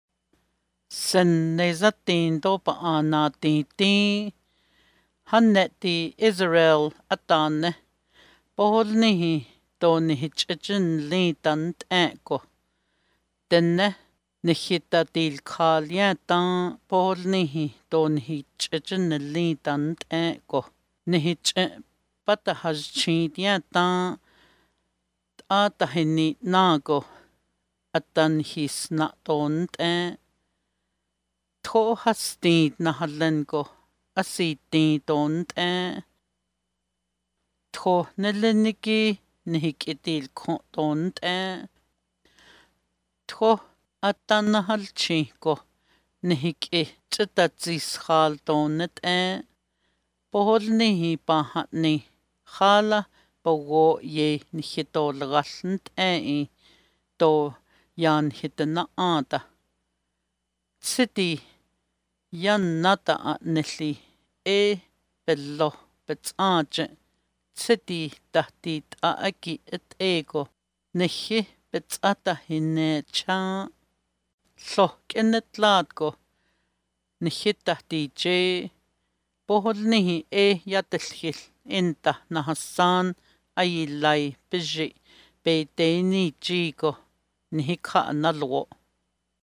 Psalm 124 - Navajo Audio Bible - Psalms in Navajo
Listen to the Bible in the Navajo Language!

Audio courtesy of Navajo Bible Recordings 2020

We invite you to explore the other Native American and First Nations language resources that we have available on our